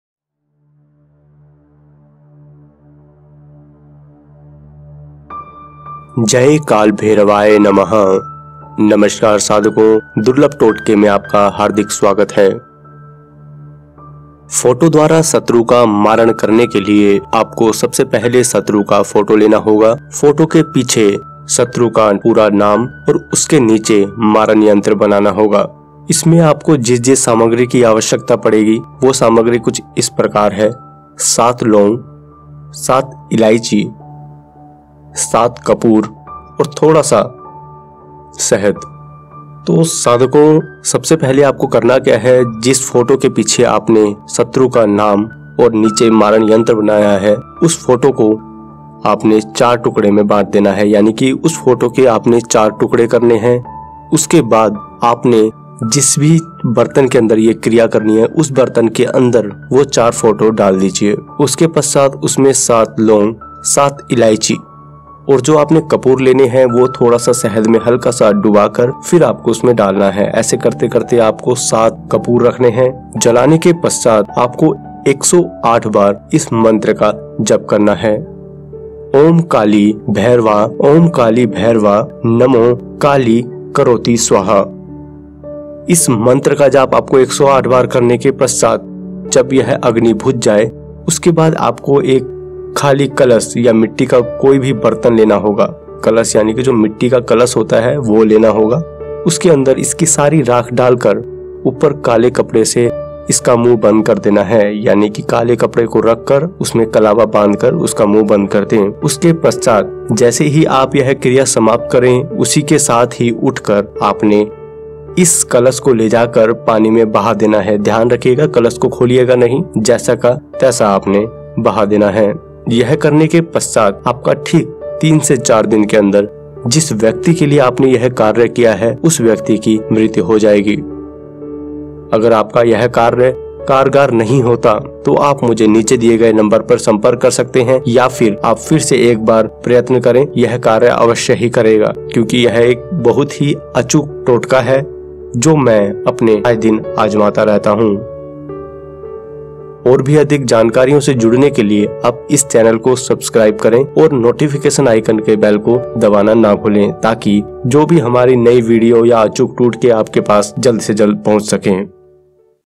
फोटो से करें शत्रु का मारण | Kill Your Enemy By Photo | दुश्मन की मृत्यु के लिए दुर्लभ उपाय
"कृपया DURLABH TOTKE{दुर्लभ उपाय } को सब्स्क्रिब करना ना भूले"

About This Video : फोटो से करें शत्रु का मारण | Kill Your Enemy By Photo | दुश्मन की मृत्यु के लिए  दुर्लभ उपाय

MANTRA : na
PHOTO-MARAN